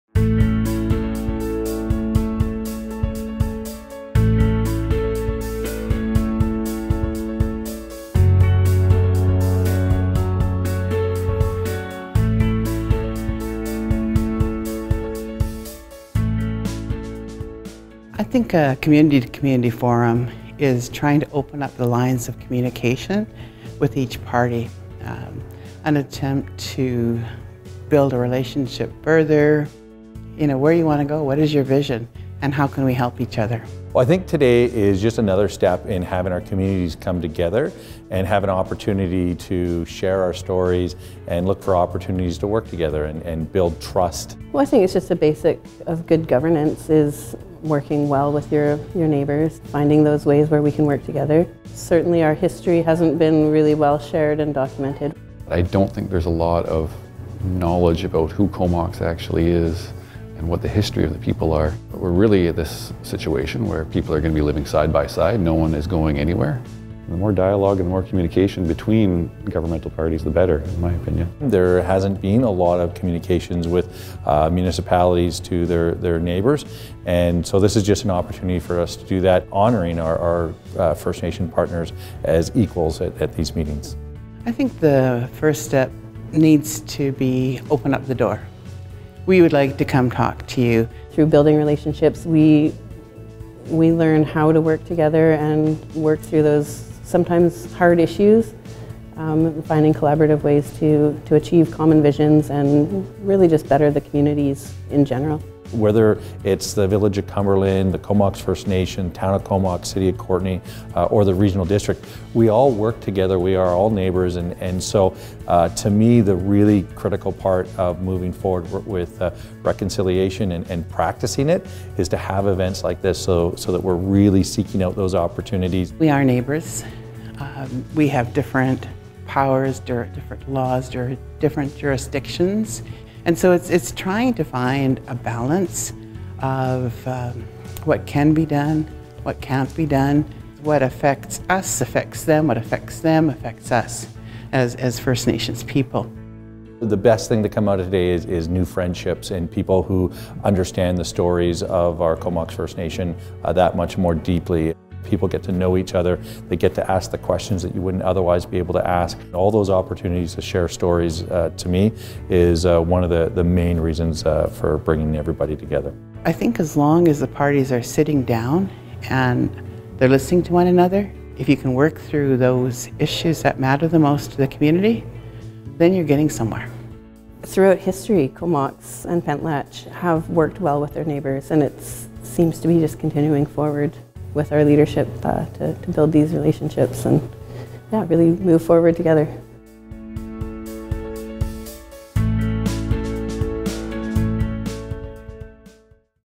KFN and CVRD Community to Community Forum
On February 26, 2019, the K’ómoks First Nation (KFN) and Comox Valley Regional District (CVRD) held their first Community to Community Meeting.